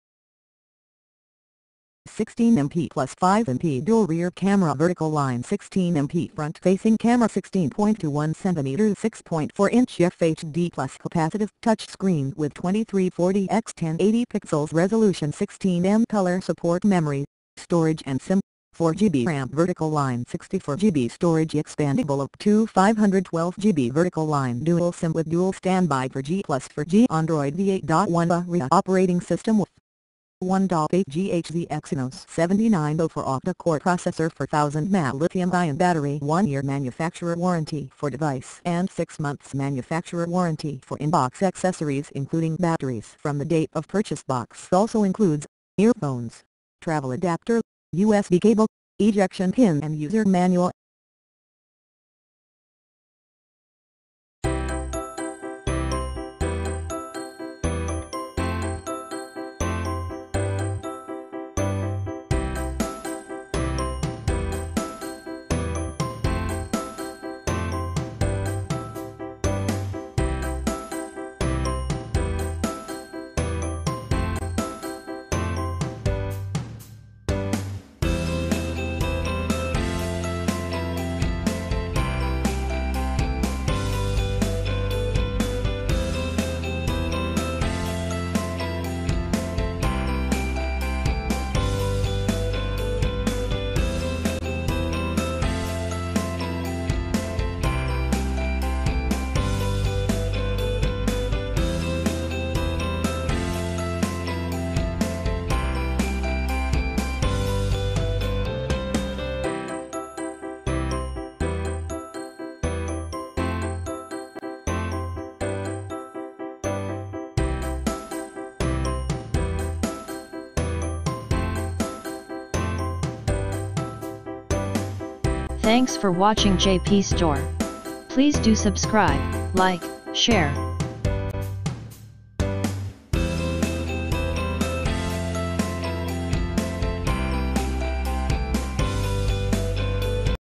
Samsung Galaxy A30 (Red, 4GB RAM, 64GB Storage) Without Offer
16MP+5MP dual rear camera with f1.7 wide angle and f2.2 flash | 16MP front camera
16.21 centimeters (6.4-inch) FHD+ multi-touch capacitive touchscreen with 2340 x 1080 pixels resolution and 16M color support
Memory, Storage and SIM: 4GB RAM | 64GB internal memory expandable up to 512GB | Dual SIM (nano+nano) dual-standby (4G+4G)
Android Pie v9.0 operating system with 1.8GHz Exynos 7904 octa core processor, CPU Type: Octa-Core
4000mAH lithium-ion battery with Type-C fast charging | 15W Charger in the box
1 year manufacturer warranty for device and 6 months manufacturer warranty for in-box accessories including batteries from the date of purchase
Box also includes: Earphones, Travel Adapter, USB Cable, Ejection Pin and User Manual